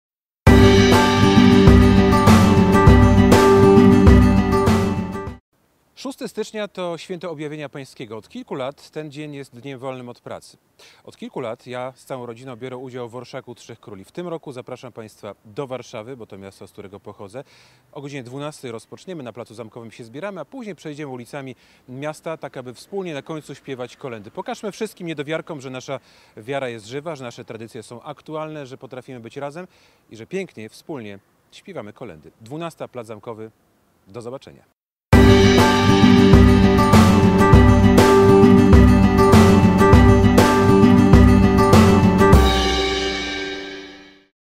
Krzysztof Ziemiec ambasadorem Orszaku Trzech Króli.
Czy Wy też lubicie śpiewać kolędy? Przyłączcie się do nas. 6 stycznia w Polsce i na świecie...

Krzysztof Ziemiec dołączył do  Ambasadorów Orszaku Trzech Króli.